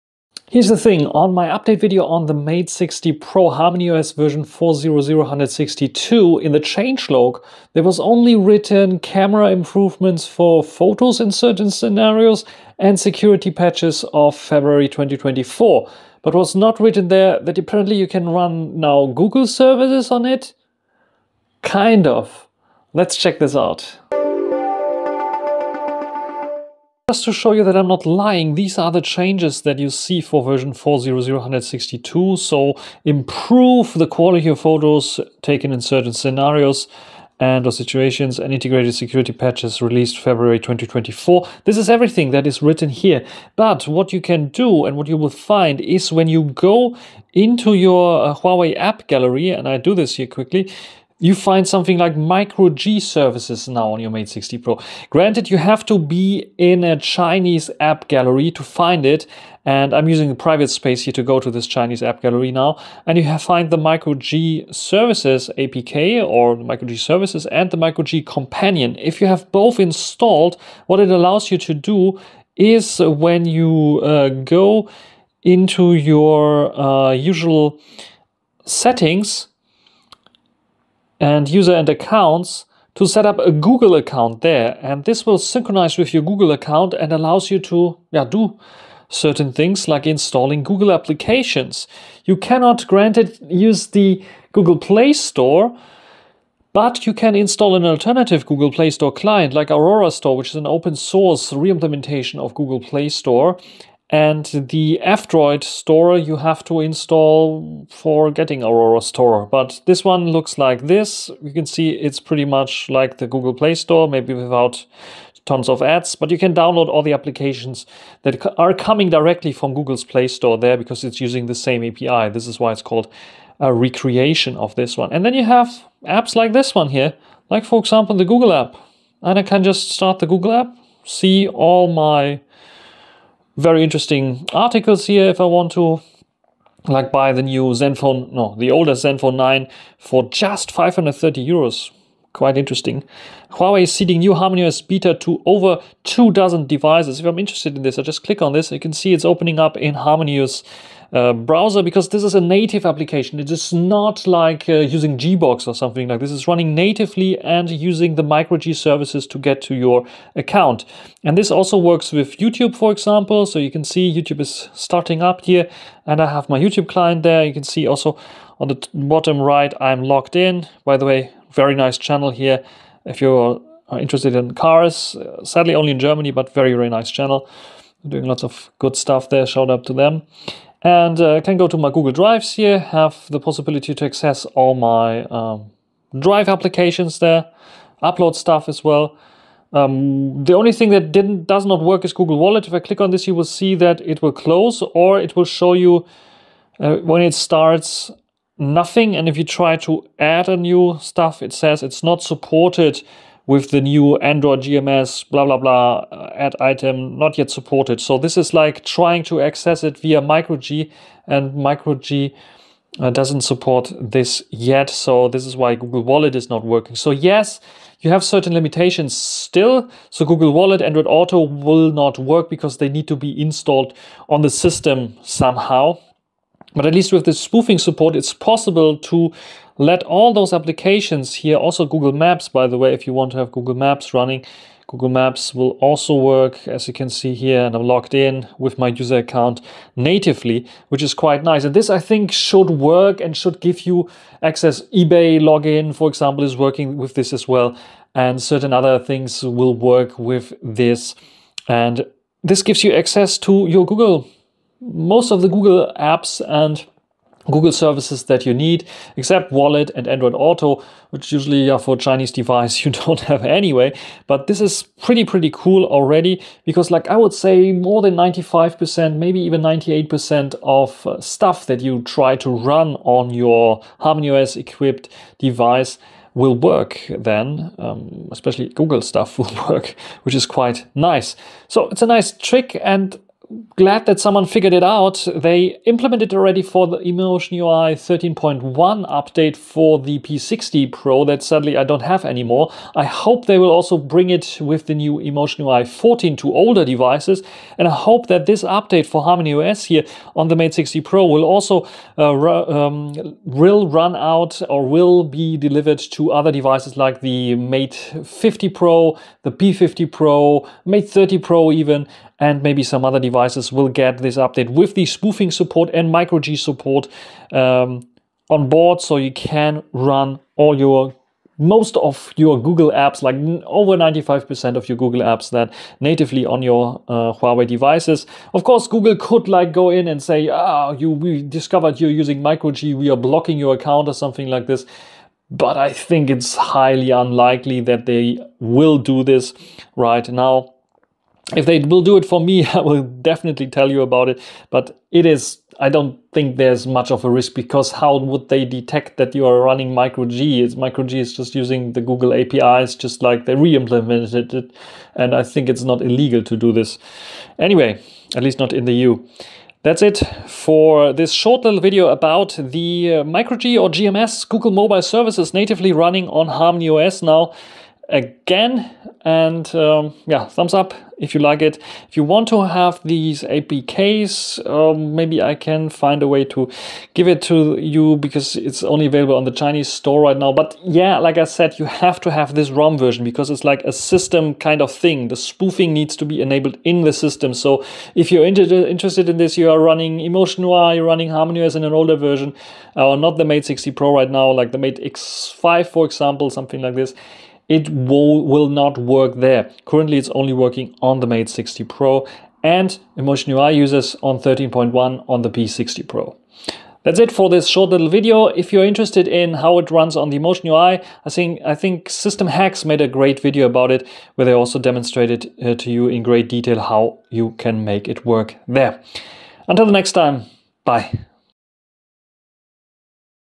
Huawei Mate 60 Pro (🇨🇳) - HarmonyOS Update allows GMS (kind of)
The latest HarmonyOS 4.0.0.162 update also features spoofing support which allows you to install microG from the chinese AppGallery or officiall website to use Google Play Services natively on your Mate 60 Pro.
In this video I show you how it looks like and what kind of limitations you will encounter.

Make sure to download the -hw versions underneath the normal ones listed in the download page.
com.google.android.gms-hw
com.android.vending-hw